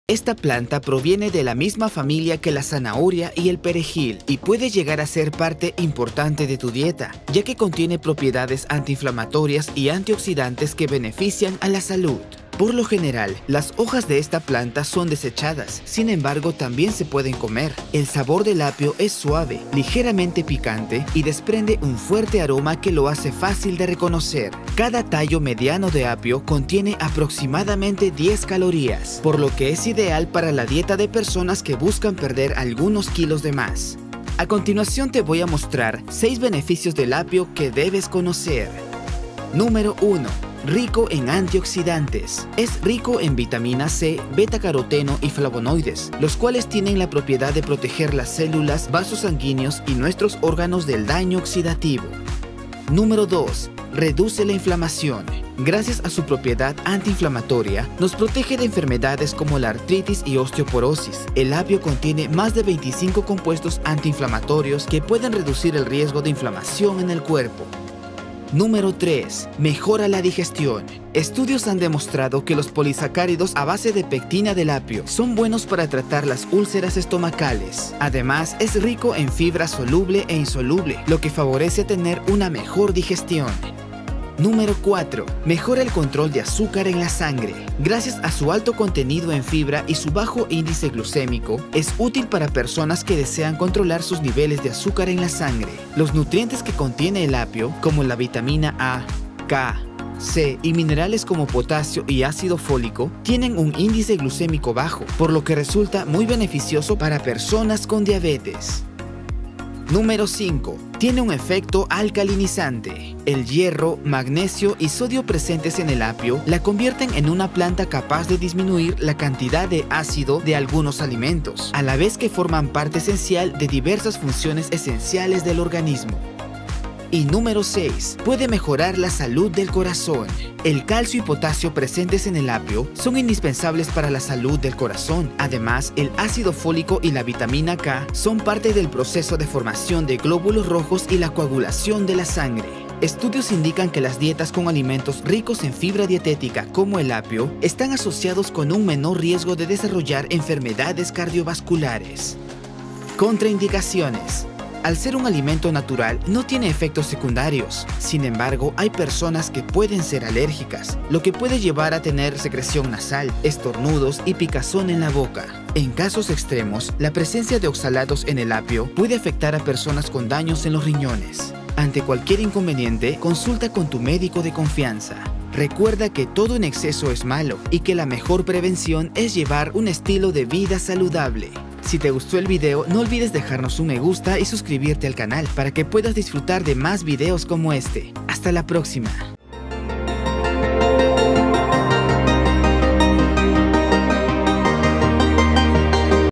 6 BENEFICIOS del APIO que DEBES CONOCER ✅
Esta planta proviene de la misma familia que la zanahoria y el perejil y puede llegar a ser parte importante de tu dieta, ya que contiene propiedades que antiinflamatorias y oxidantes que benefician a la salud.

Por lo general, las hojas de esta planta son desechadas, sin embargo, también se pueden comer. El sabor del apio es suave, ligeramente picante y desprende un fuerte aroma que lo hace fácil de reconocer.

Cada tallo de apio mediano contiene aproximadamente 10 calorías, por lo que es ideal para la dieta de personas que buscan perder algunos kilos de más.

¿Quieres saber mas? Te recomiendo ver este video donde te voy a presentar 6 beneficios del apio PARA LA SALUD ✅